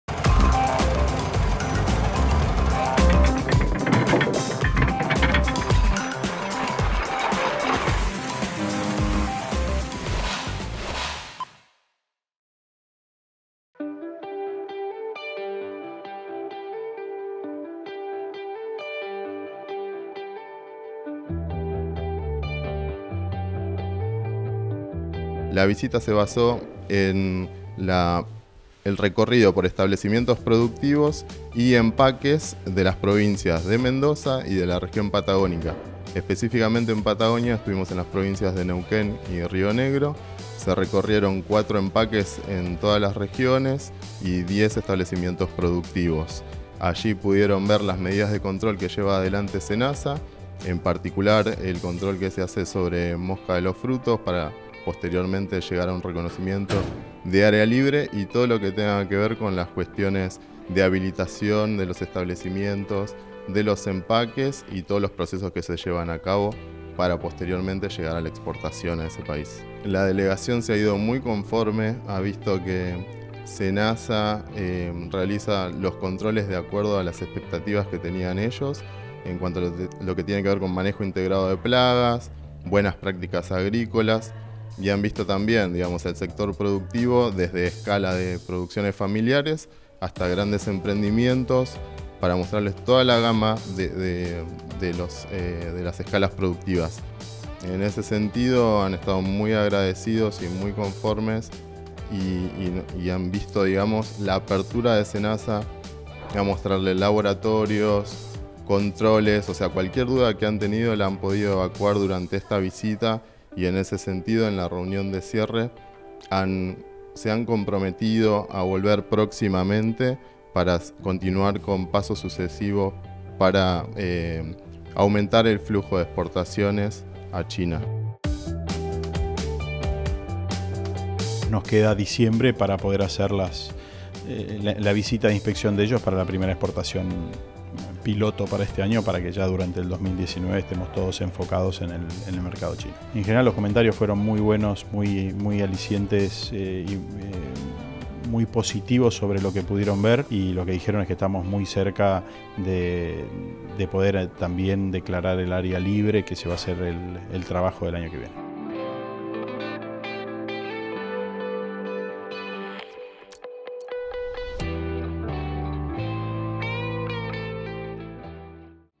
Protocolo fitosanitario para exportar cerezas a China
Los presidentes de la Argentina y China firmaron en Buenos Aires, el documento que fue acordado previamente  por el Senasa y la Aduana del país de Oriente.